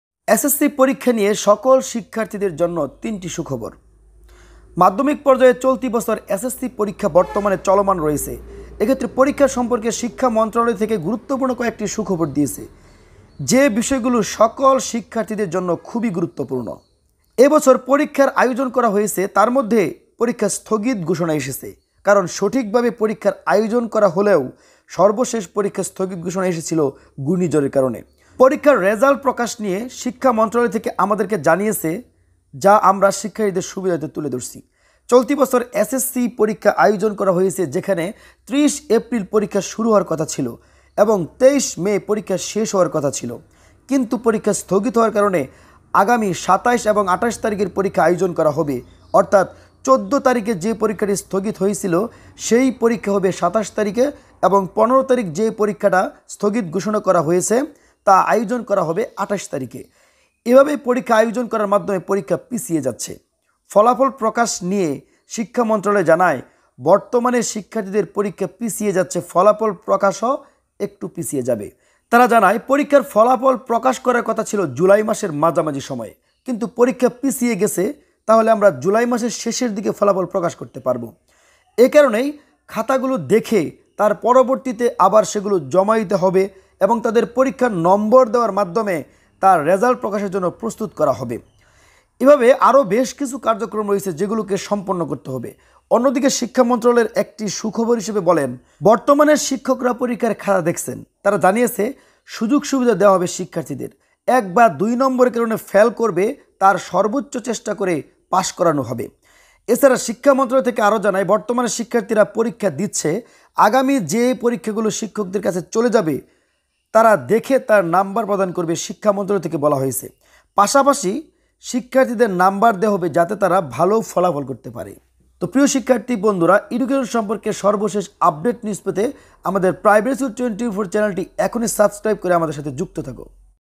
২০২৩ সালের এসএসসি পরীক্ষার্থীদের জন্য আবারো ৩টি সুখবর।।Good news for the students of SSC exam 2023।।
২০২৩ সালের এসএসসি পরীক্ষার্থীদের জন্য আবারো ৩টি সুখবর।।Good news for the students of SSC exam 2023